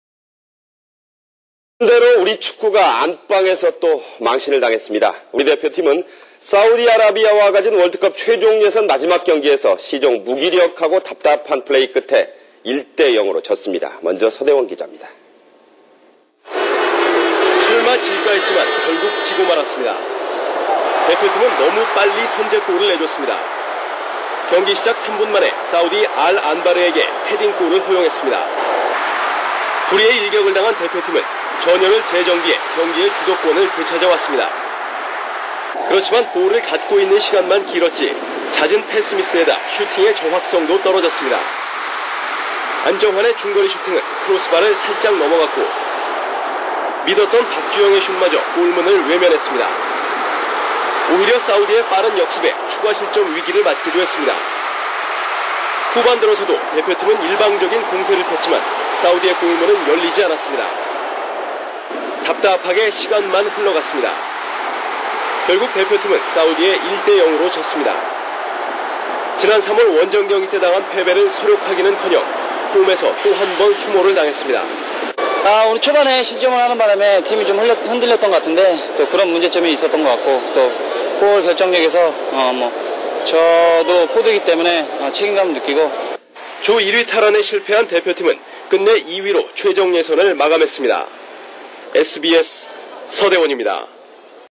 2005/8/17 (World Cup Qualifier) South Korea 0 Saudi Arabia 1
Korean Commentary
Goal at 0:20 (Al-Anbar)